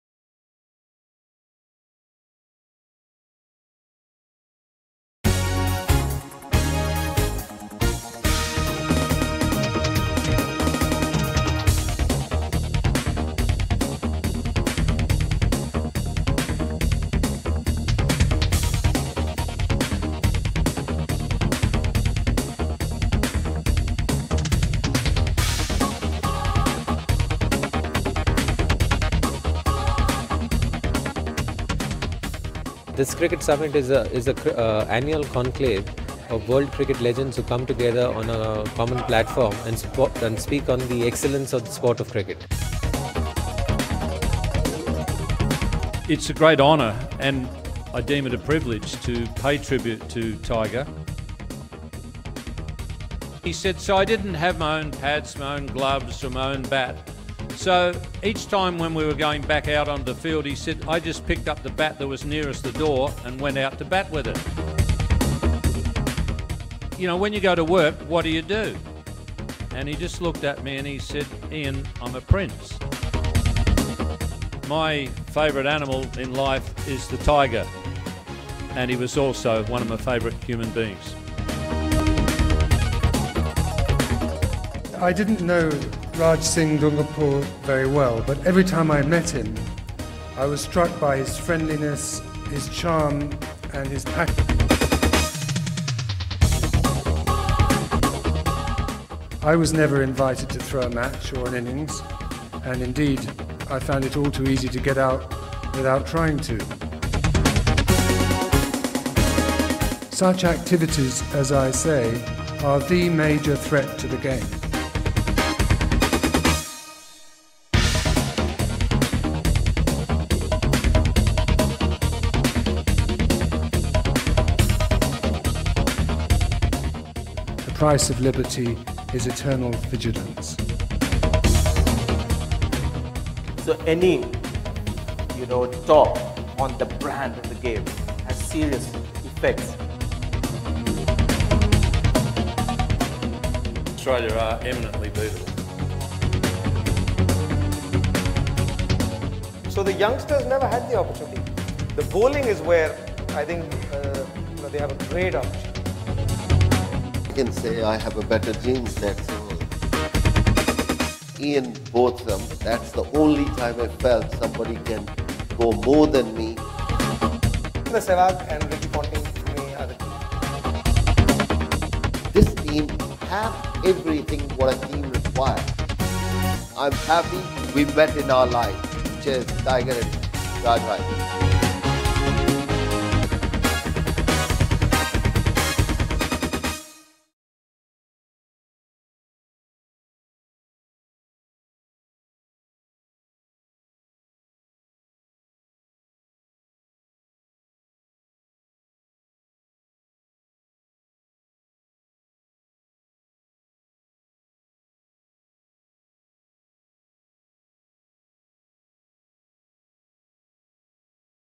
Raj Singh Dungarpur World Cricket Summit, Mumbai
BBH proposed and initiated a fitting tribute to Nawab Mansur Ali Khan of Pataudi at the Raj Singh Dungarpur World Cricket Summit by flying in Ian Chappell from Australia who address the Nawab as a lovable larrikin and said, Tiger was amongst his favourite people. It was a memorable evening with Mumbai celebrities. Many cricketing legends addressed it as one of the finest cricketing evening of the world.